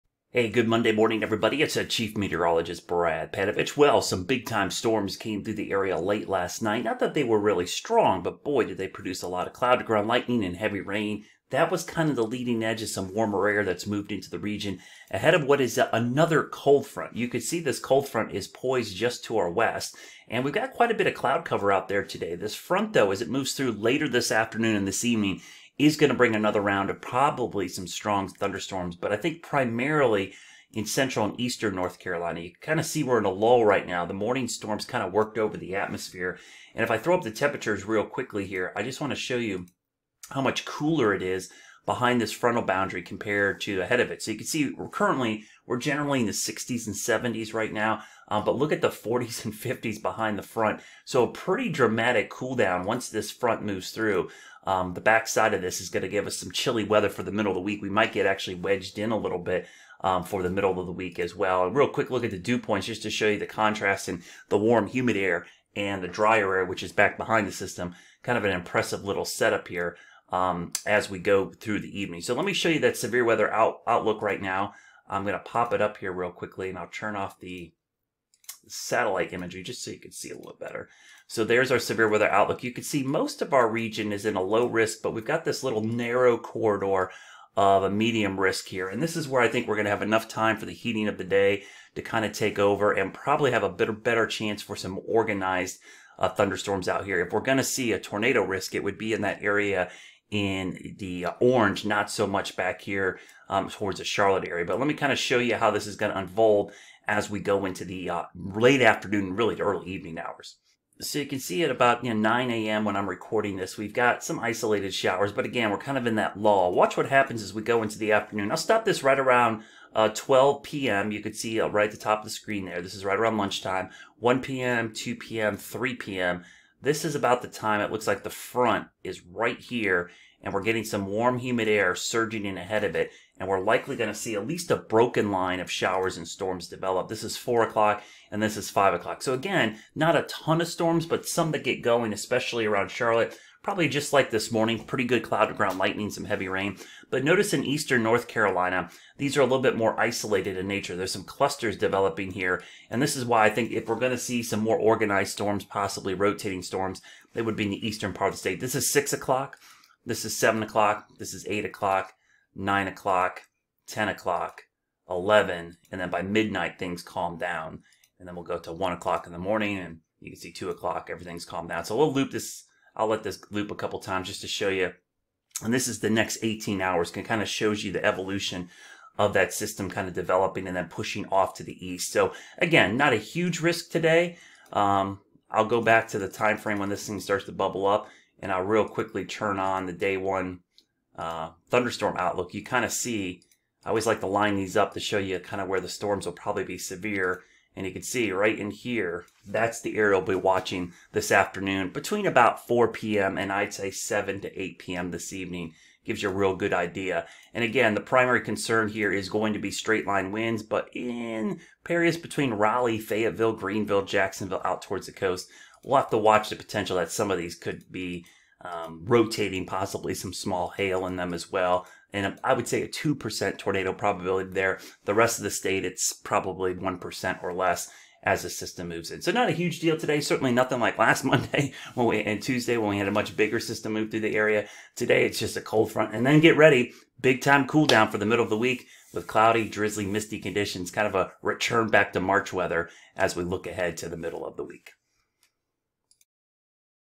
Monday WX VLOG 5/10/2021: Scattered strong storms possible this evening.
Monday WX VLOG 5/10/2021: Scattered strong storms possible this evening. A cold front moves through the area tonight and we could see some strong storms form along the front.